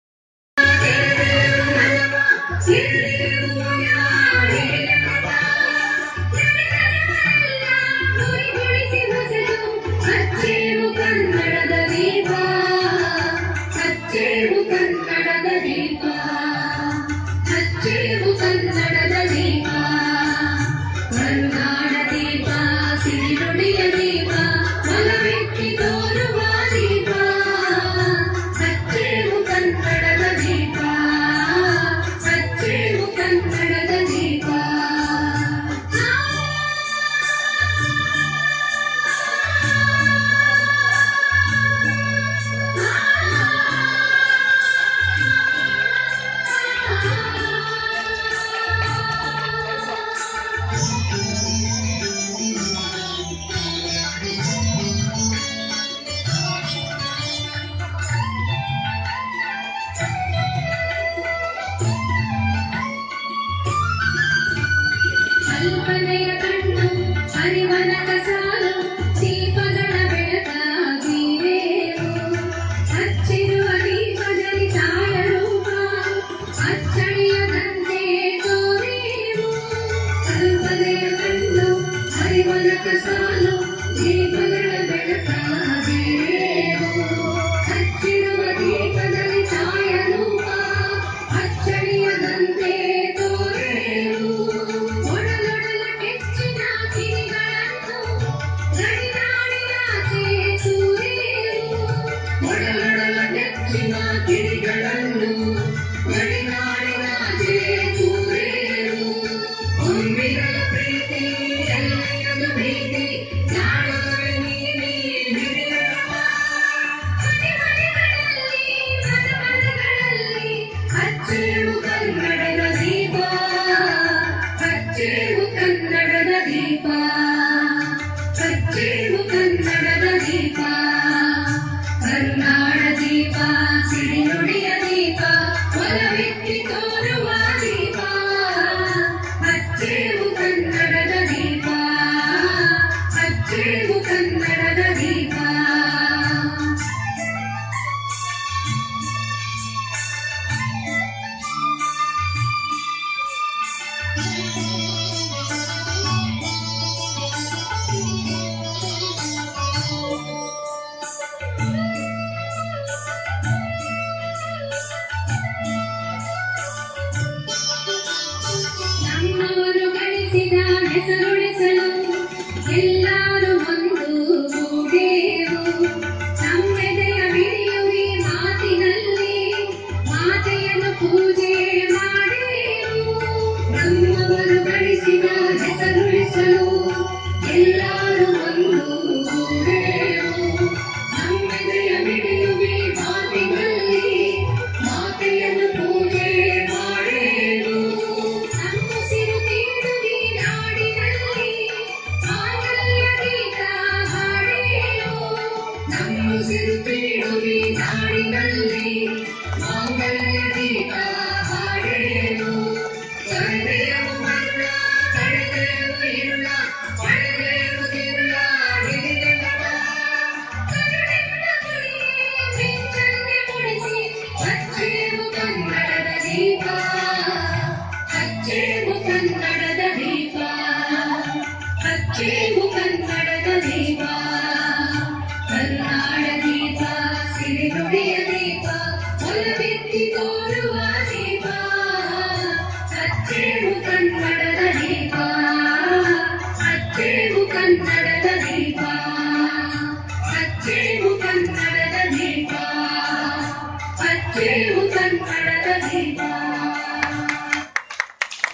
Children's Cultural Festival - 2016
This is the dance performance by Students of Chembur Karnatak High School for "Children's Cultural Festival - 2016" at Mysore Association, Mumbai.